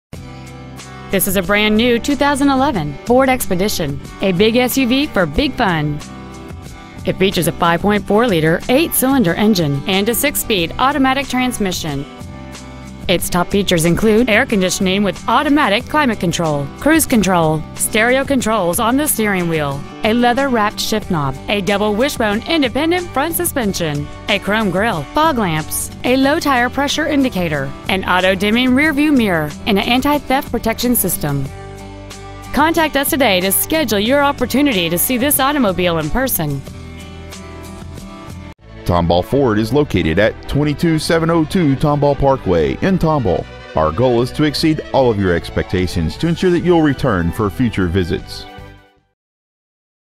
2011 Ford Expedition Tomball TX
Year: 2011
 Make: Ford
 Model: Expedition
 Engine: 5.4L SOHC 24-VALVE V8 FFV ENGINE
 Trans.: automatic
 Exterior: White Platinum Metallic Tri-Coat
 Miles: 3
 Interior: Charcoal Black w/King Ranch Red
 Stock: